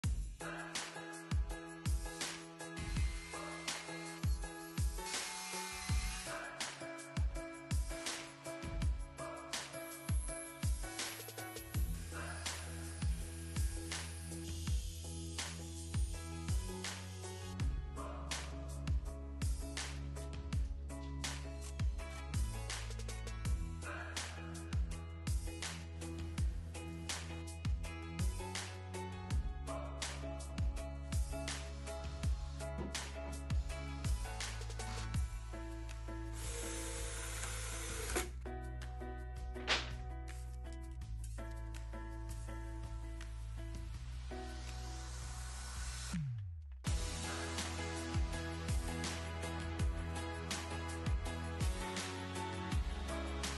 How to build an EASY bed platform
We needed a bed platform for our guest room, so I built one! I used easily accessible materials from the big box store, and I have build plans available below if you'd like to build one too. One of my favorite DIY builds. Check out the full video below.